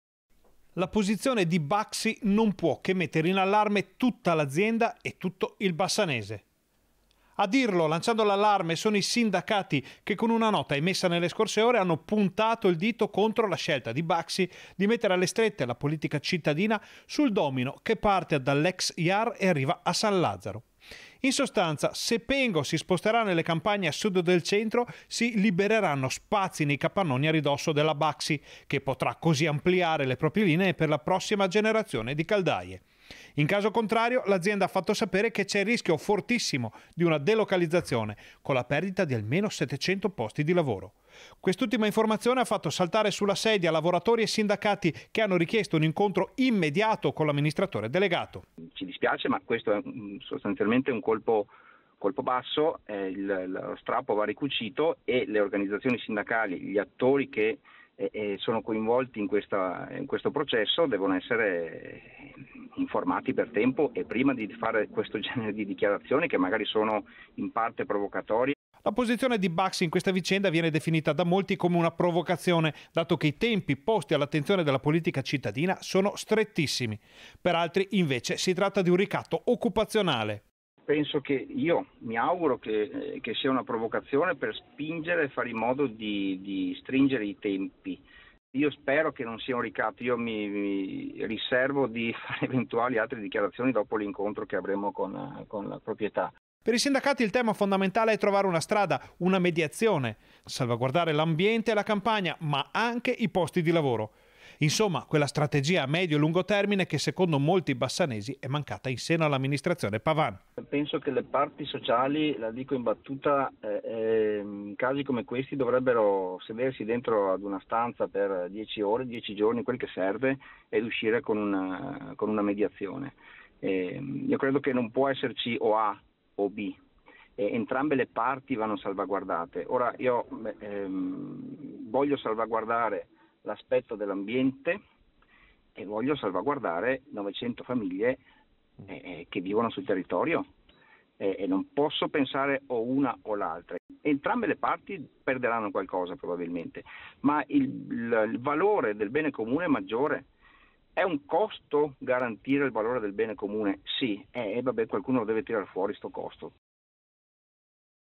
SINDACATI SUL PIEDE DI GUERRA PER IL CASO BAXI: «SPERIAMO SIA UNA PROVOCAZIONE» | 18/05/2023
TG Bassano del Grappa 18/05/2023 - BASSANO DEL GRAPPA - Dopo la lettera inviata da Baxi al sindaco e ai consiglieri comunali di Bassano ora i sindacati pretendono una riunione con l'azienda per capire cosa stia accadendo e quali siano le prospettive sui posti di lavoro || La posizione di Baxi non può che mettere in allarme tutta l’azienda e tutto il bassanese. A lanciare l’allarme sono i sindacati, che con una nota emessa nelle scorse ore hanno puntato il dito contro la scelta della Baxi di mettere alle strette la politica cittadina sul domino che parte dall’ex Iar e arriva a San Lazzaro: in sostanza se Pengo si sposterà nelle campagne a sud del centro si libereranno spazi nei capannoni a ridosso della Baxi, che potrà così ampliare le proprie linee per la prossima generazione di caldaie. In caso contrario l’azienda ha fatto sapere che c’è il rischio fortissimo di una delocalizzazione, con la perdita di almeno 700 posti di lavoro. Quest’ultimo dettaglio ha fatto saltare sulla sedia i lavoratori e i sindacati, che hanno richiesto un incontro immediato con l’amministratore delegato. La posizione di Baxi in questa vicenda viene definita da molti come una provocazione, dato che i tempi posti all’attenzione della politica cittadina sono strettissimi. Per i sindacati il tema fondamentale è trovare una strada, una mediazione. Salvaguardare l’ambiente e la campagna, ma anche i posti di lavoro. Insomma, quella strategia a medio e lungo termine che secondo molti bassanesi è mancata in seno all’Amministrazione Pavan. - Intervistati Al telefono DAVIDE PASSUELLO (Fim Cisl ) (Servizio di Ferdinando Garavello) - Segui Rete Veneta anche sul digitale terrestre! Visita il sito